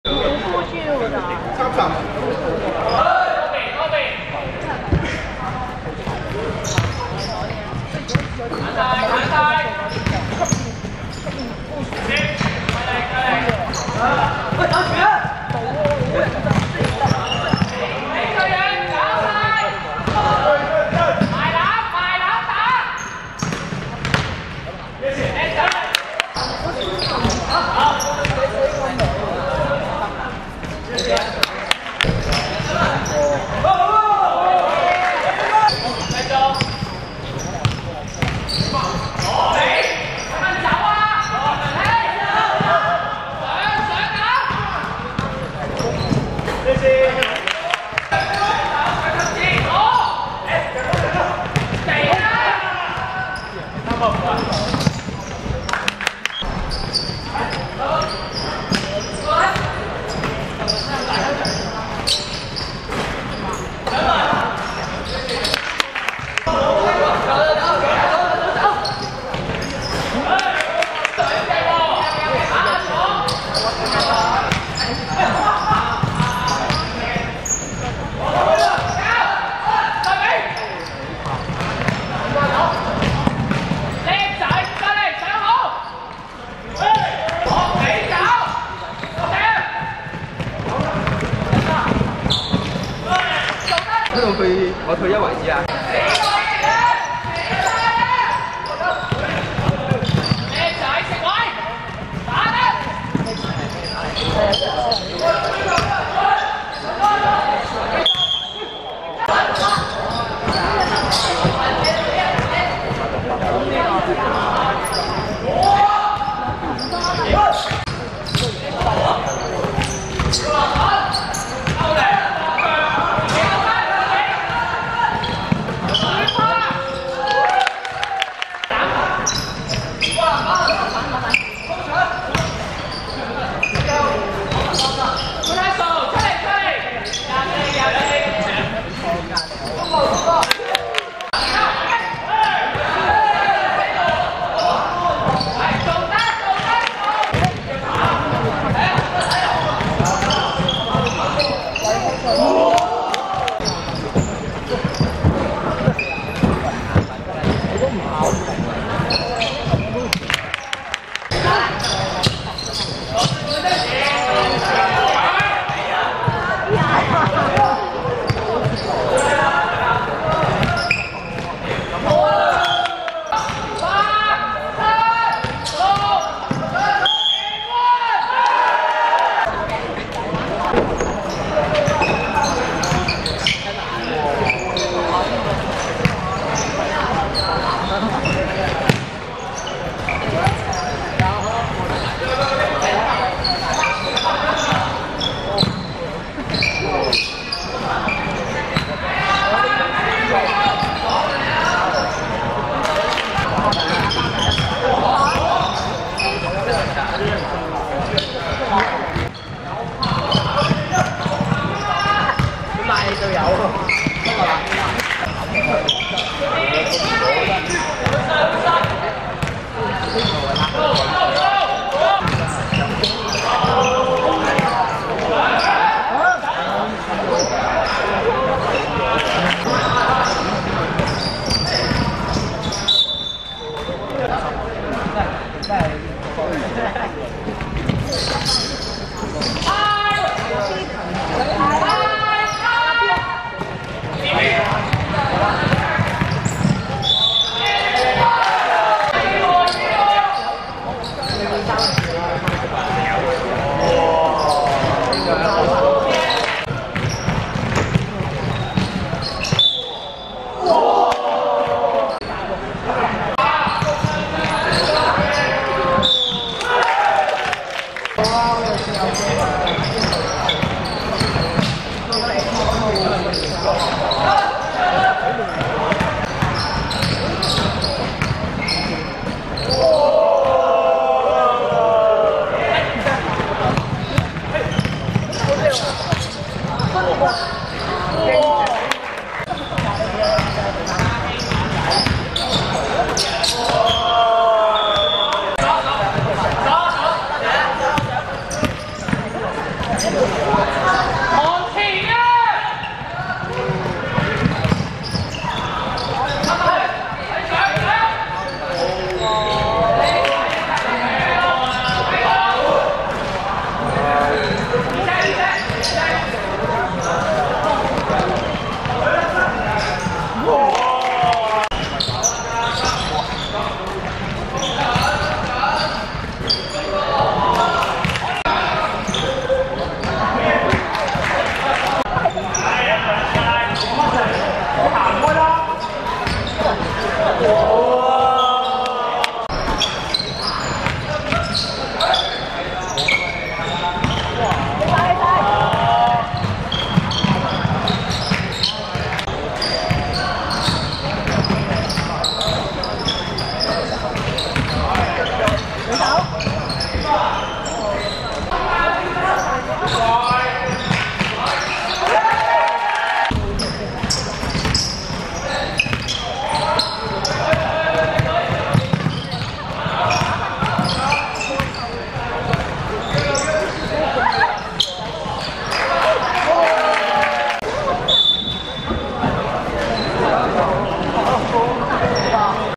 翁祐vs青年會(2018.11.6.元朗學界男子籃球乙組)精華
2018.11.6.元朗體育館 17:00 翁祐(白) 9 : 70 青年會(藍)